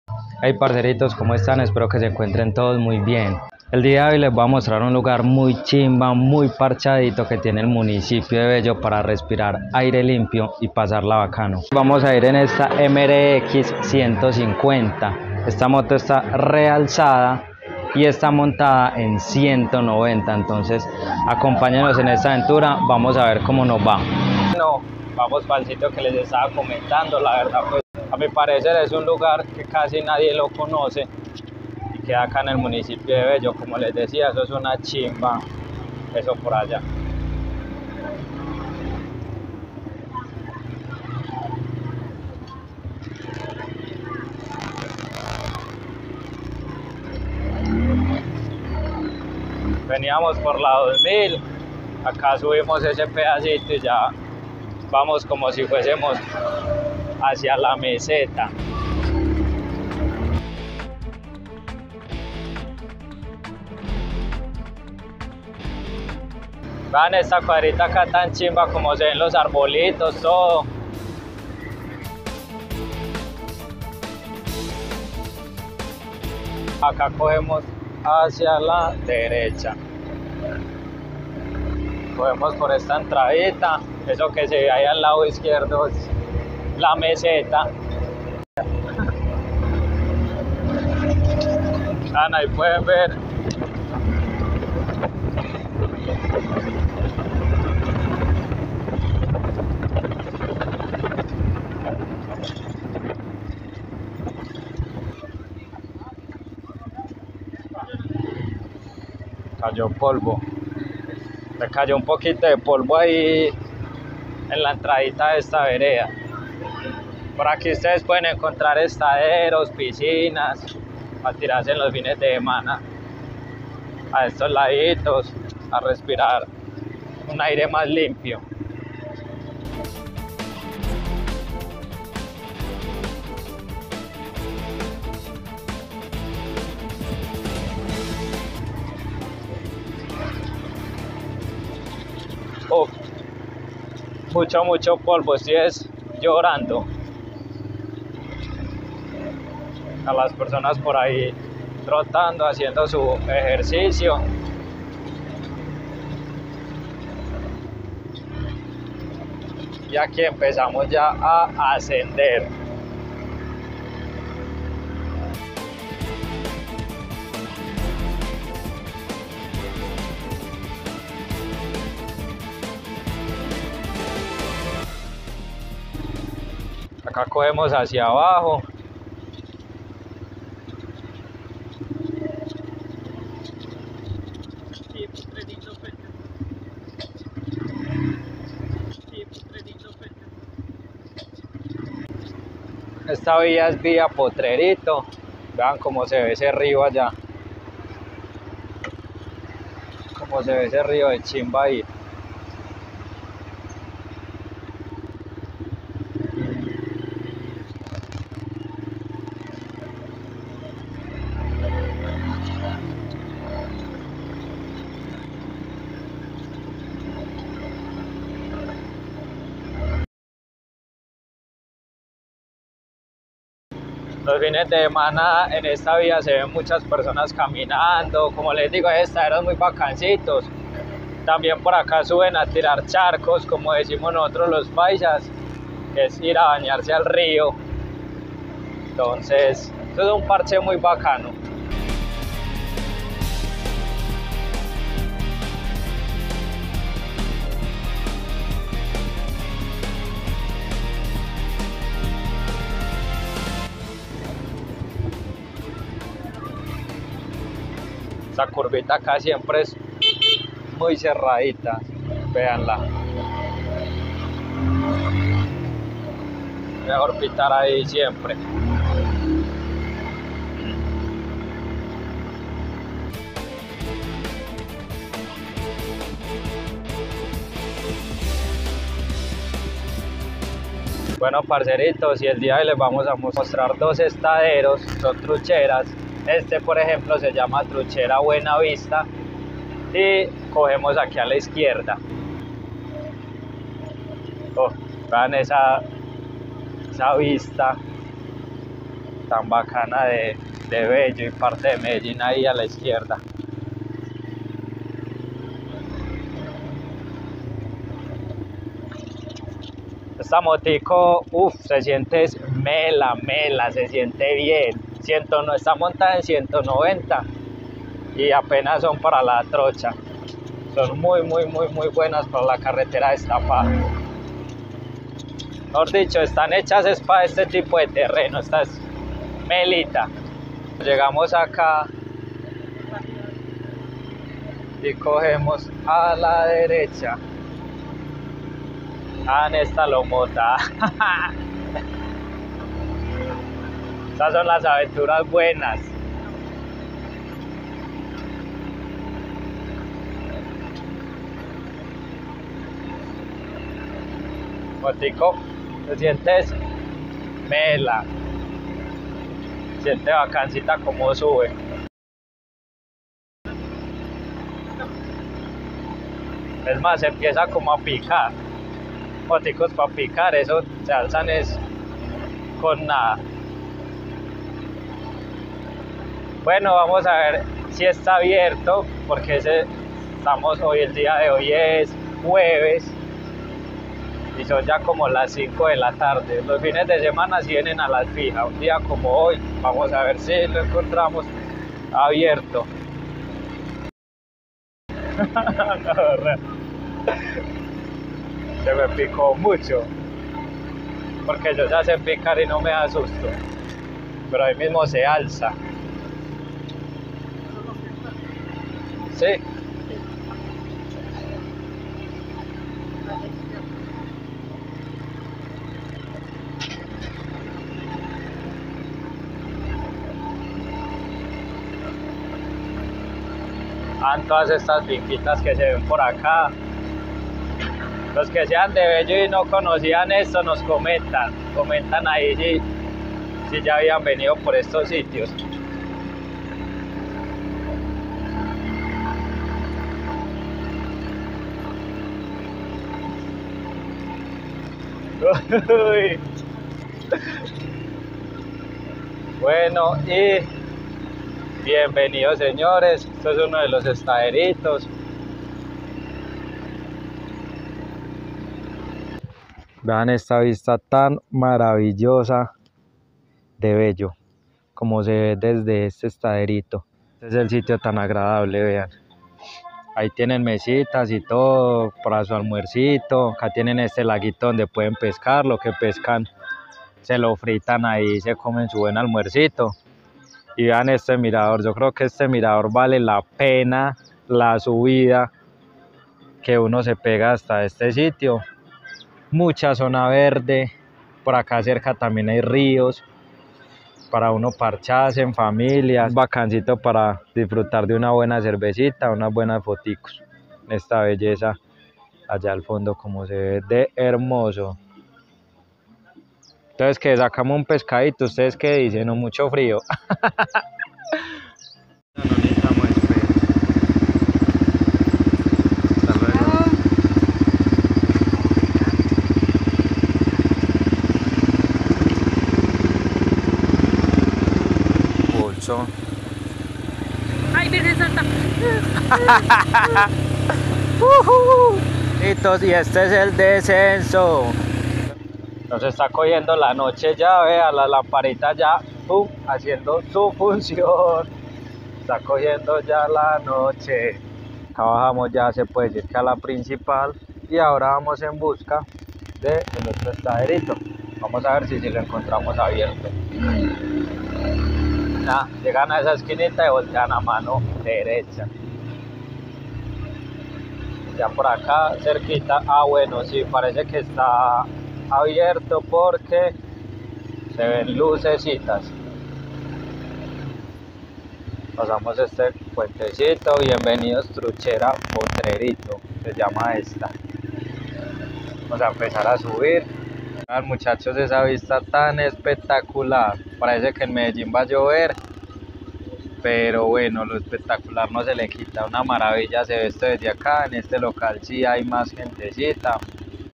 Visite una VEREDA ubicada en BELLO ANTIOQUIA
Parceritos espero que todos se encuentren muy bien, en este video recorro una vereda ubicada en el municipio de bello, esta vez hacemos el recorrido en una moto MRX 150 de VICTORY